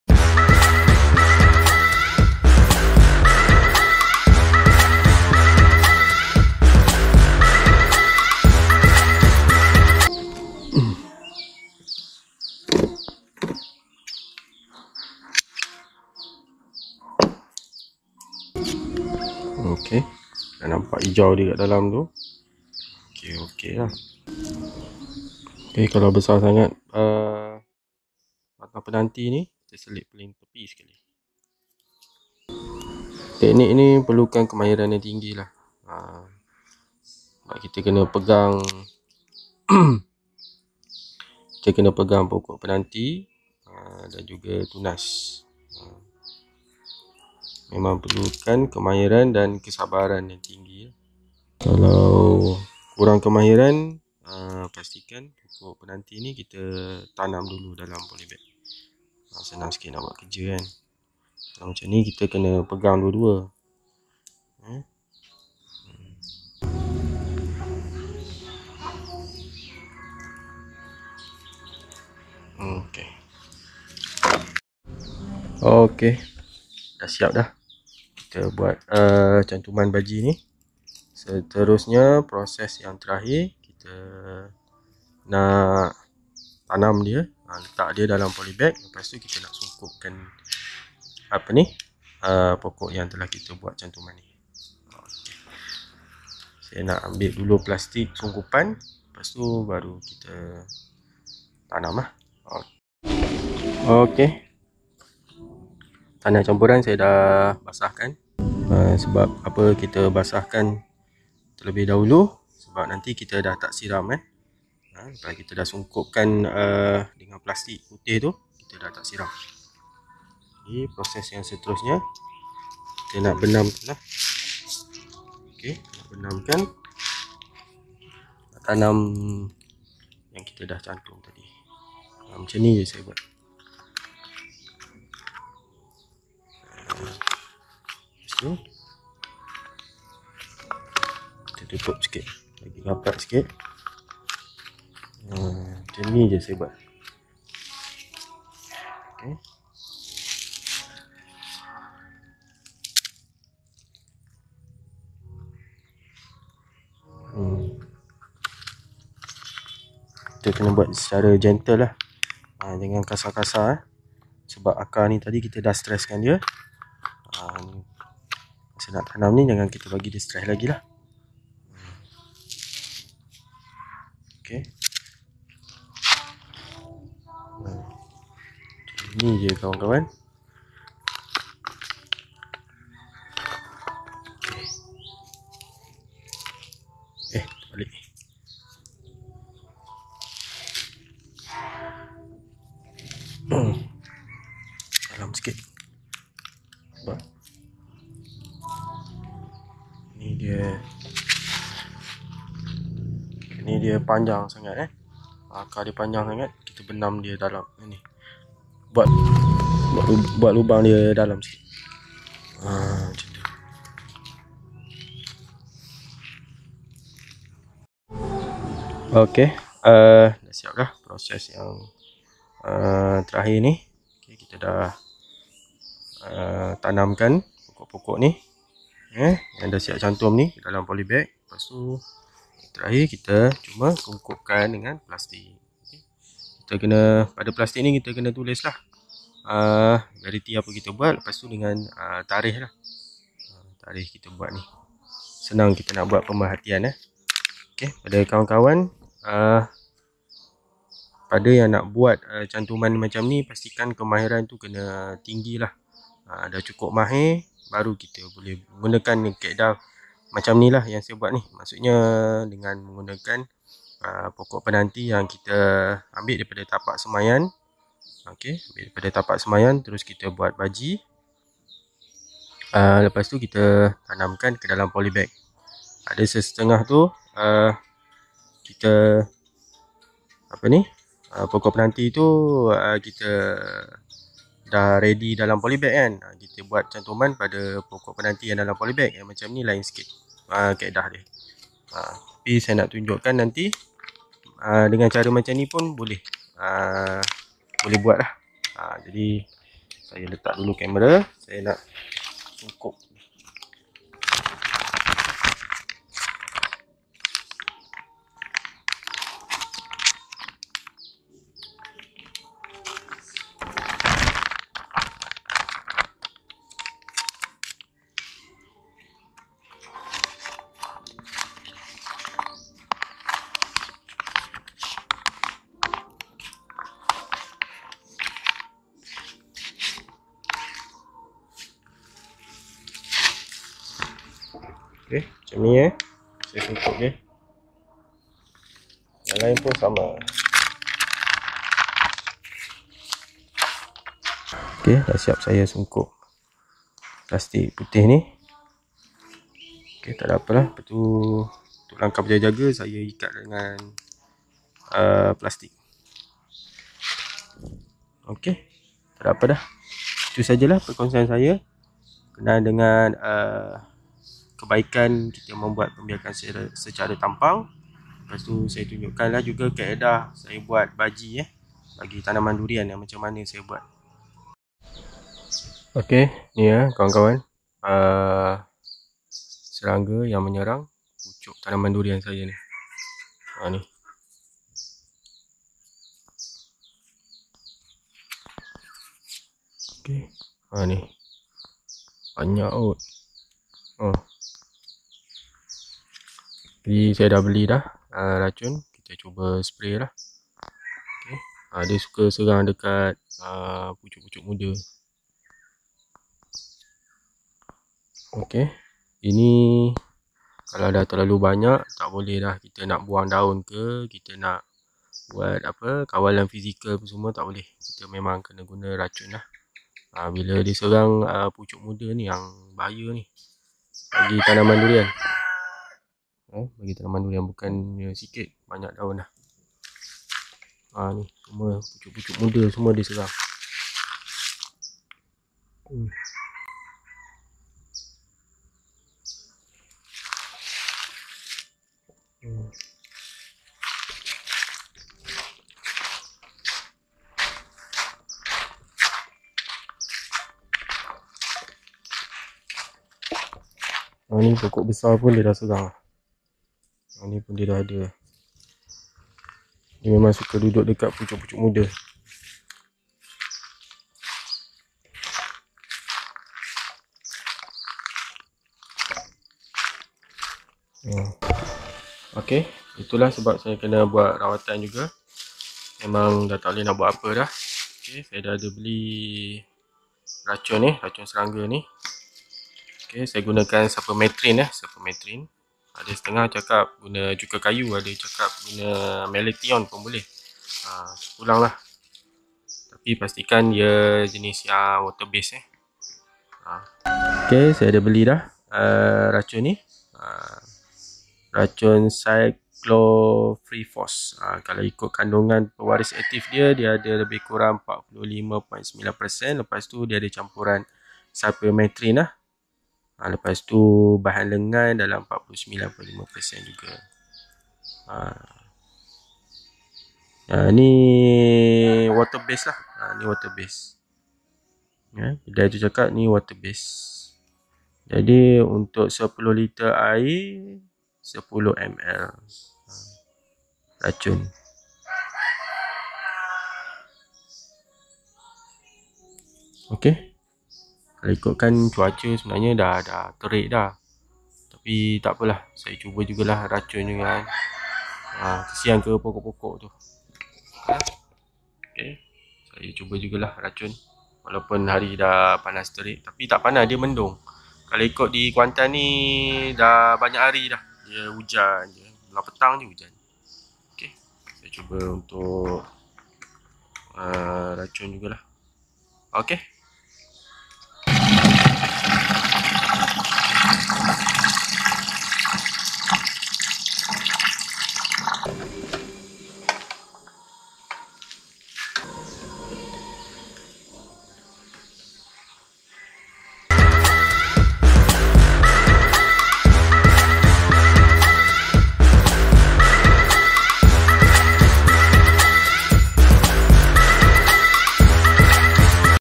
Meracun Tanaman Durian diserang kutu psyllid
Ada 1 lagi video sambungan dari video ini.
Video ini saya tunjukkan proses terakhir cantuman baji bagi pembiakan tampang yang saya lakukan.
Dan juga aktiviti meracun tanaman yang diserang serangga iaitu kutu psyllid. serangga ini suka menyerang pucuk muda tanaman durian.